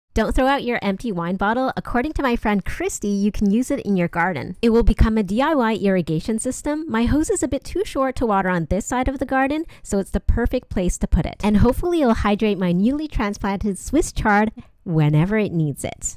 keep it for the garden 🍾
Upcycling at its best! My trusty old wine bottle has found a new purpose in my garden. 🌱🍾 With my hose being too short to reach this side of the garden, this repurposed bottle is stepping in as a handy DIY irrigation solution. Not only does it save my plants from dehydration, but it adds a touch of eco-friendly charm to my garden setup. Who knew a wine bottle could be so resourceful?